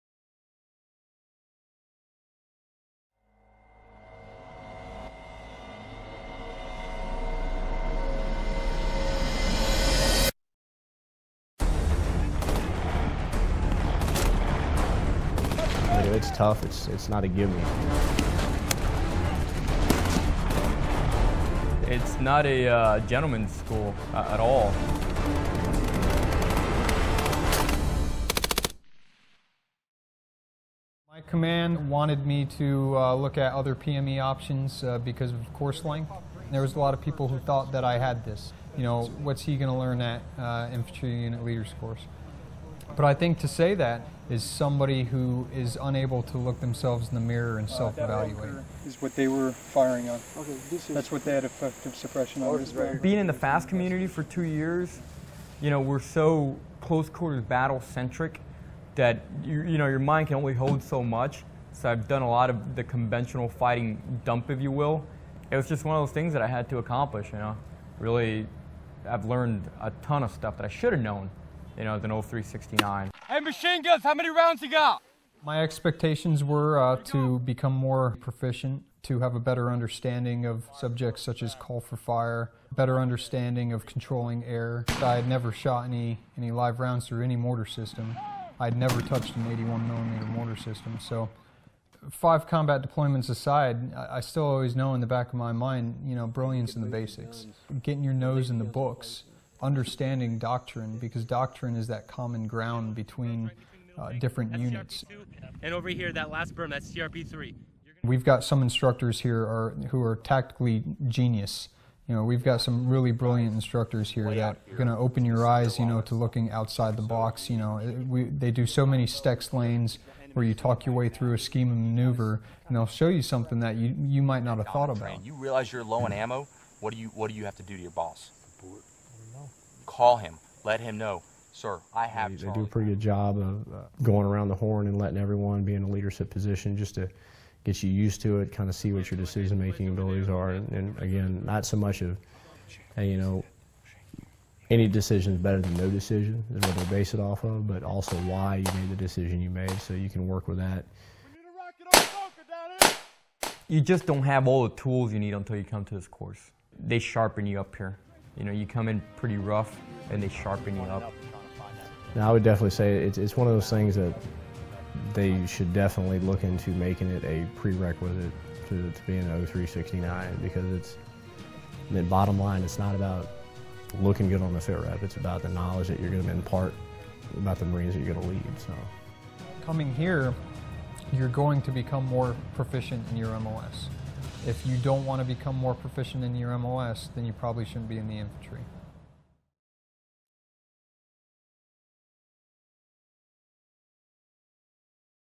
Marine Corps Infantry Unit Leaders Course
October 2013 - The Infantry Unit Leaders Course, offered through the Advanced Infantry Training Battalion, School of Infantry-East is responsible for providing instruction and training to the future Marine Corps infantry platoon sergeants. Here is a unique view through the perspective of current students.

U.S. Marine Corps video by Lance Cpl. Eric Moreno

Edited by
USA Patriotism!